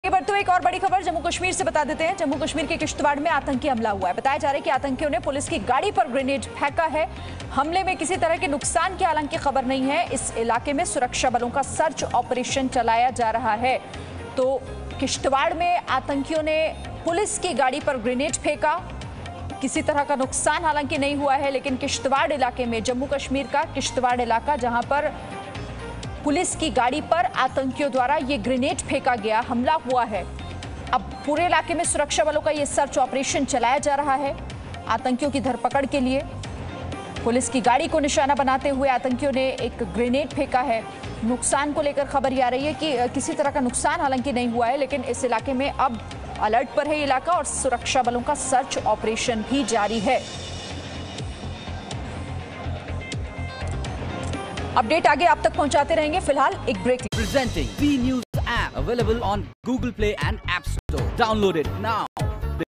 Breaking News: Jammu-Kashmir के Kishtwar में हुआ Terrorist Attack, पुलिस की गाड़ी पर Grenade से अटैक
The suspected militants threw a grenade on a police car in Kishtwar district of Jammu and Kashmir on Thursday, but they missed the target and the bomb exploded on an empty stretch of road.

संदिग्ध आतंकवादियों ने गुरुवार को जम्मू-कश्मीर के किश्तवाड़ जिले में एक पुलिस की गाड़ी पर ग्रेनेड फेंका, लेकिन वे लक्ष्य से चूक गए और बम सड़क के खाली हिस्से में विस्फोट हो गया।